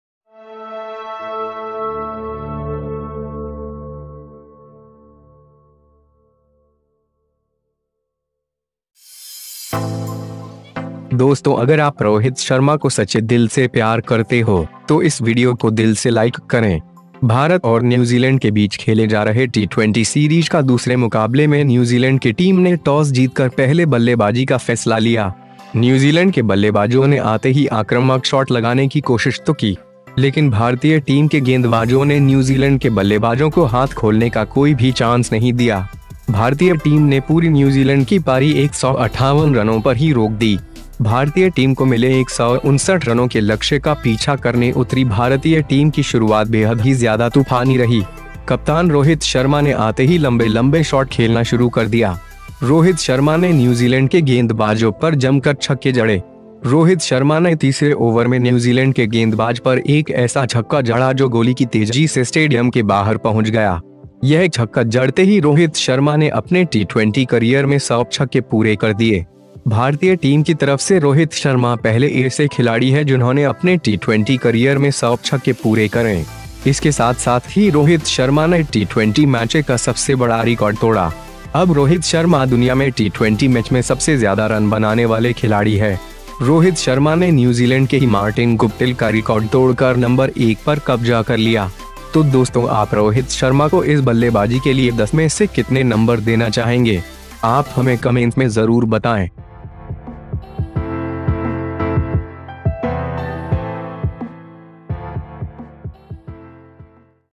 दूसरे T20 हिटमैन रोहित शर्मा ने खेली ऐतिहासिक पारी भारत का किया नाम रोशन
कप्तान रोहित शर्मा ने आते ही लंबे लंबे शॉट खेलना शुरू कर दिया . रोहित शर्मा ने न्यूजीलैंड के गेंदबाज पर जमकर छक्के जड़े . रोहित शर्मा ने तीसरे ओवर में न्यूजीलैंड के गेंदबाज पर एक ऐसा छक्का जड़ा जो गोली की तेजी से स्टेडियम के बाहर पहुंच गया . यह छक्का जड़ते ही रोहित शर्मा ने अपने t twenty करियर में 100 छक्के पूरे कर दिए . भारतीय टीम की तरफ से रोहित शर्मा पहले एसे खिलाड़ी है जिन्होंने अपने t twenty करियर में 100 छक्के पूरे करें .